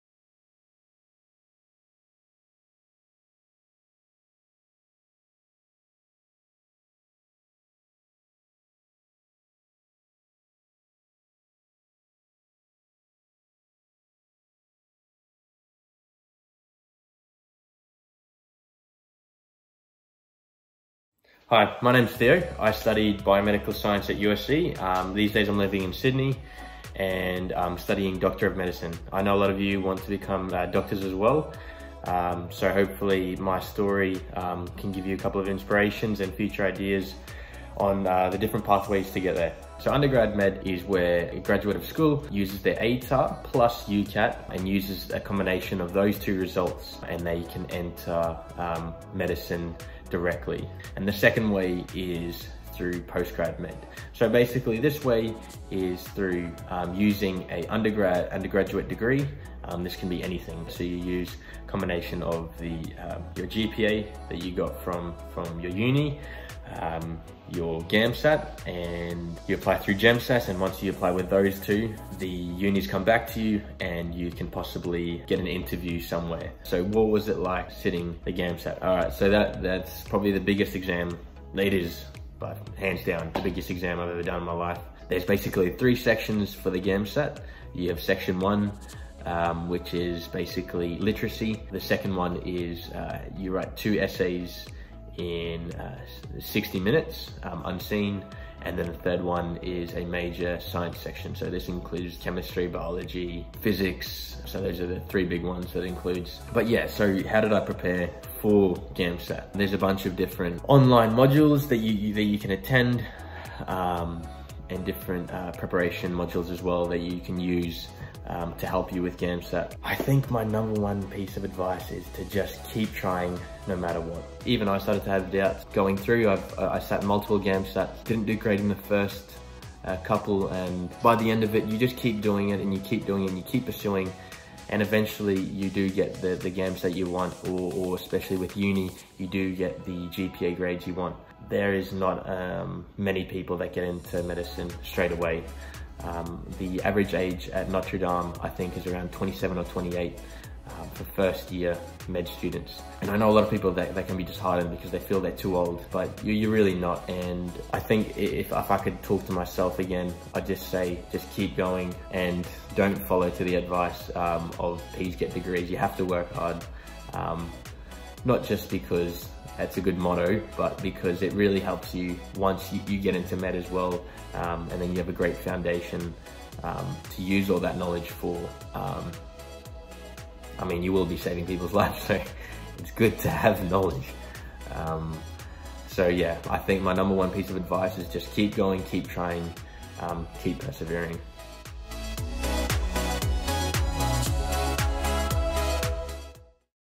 Pathways into Medicine: Theo's story
USC Biomedical Science graduate Theo talks about his pathway to studying graduate medicine.